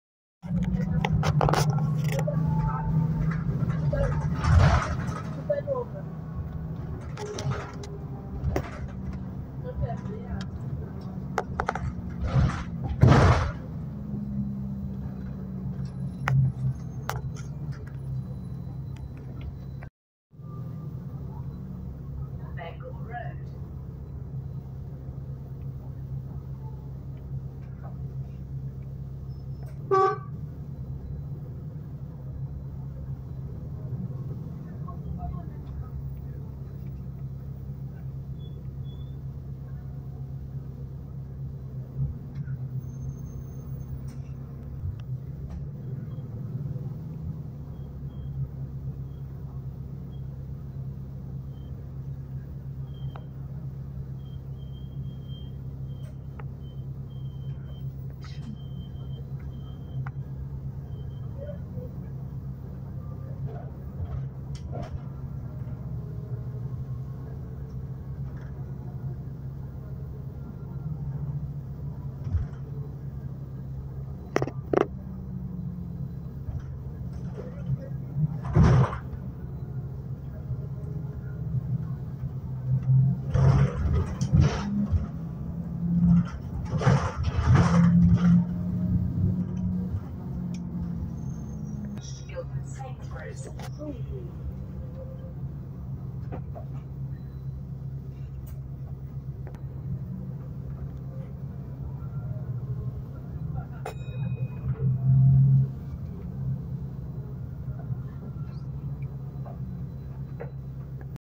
Short Journey Route EL1 East London Transit (LT400)
Speedy Driver On LT400.